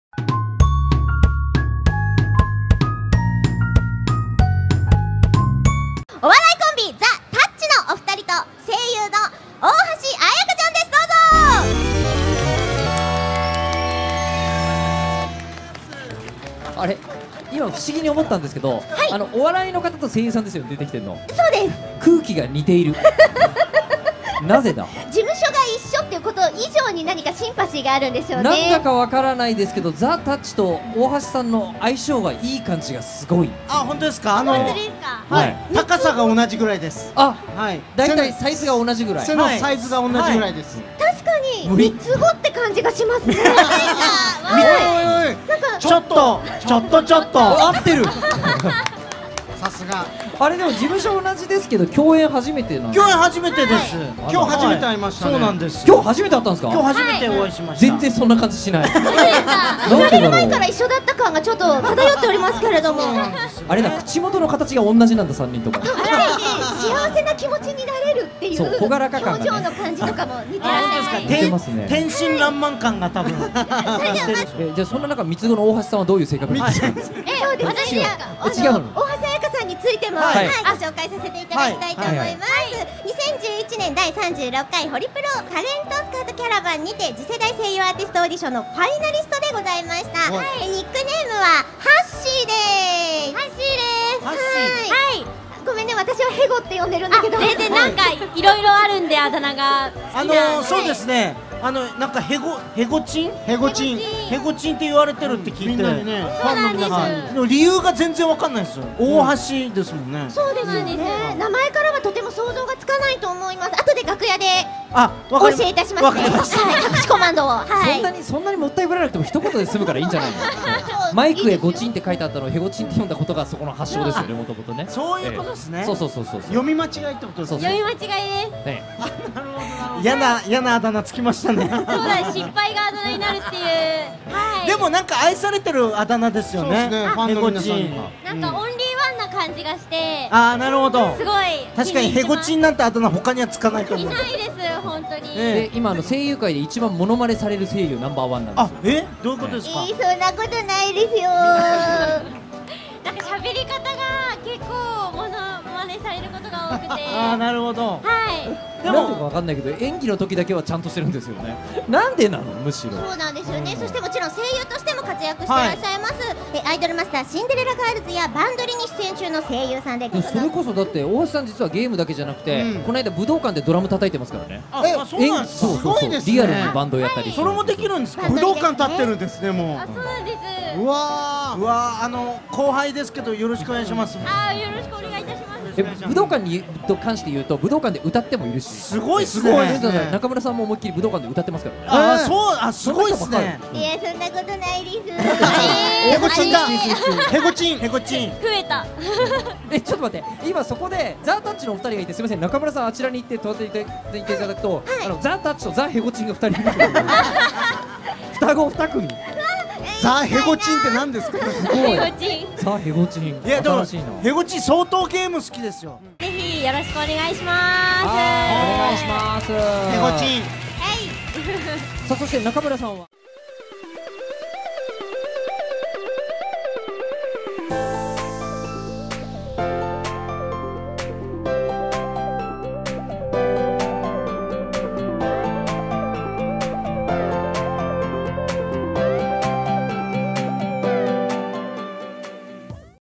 アイドルマスター大先輩声優の中村繪里子から「へご」と呼ばれてものまねされる大橋彩香ww
大橋彩香
中村繪里子

アイドルマスター
シンデレラガールズ
Idolmaster
Idolm@ster
へごちん
へご
ザ・たっち
吉田尚記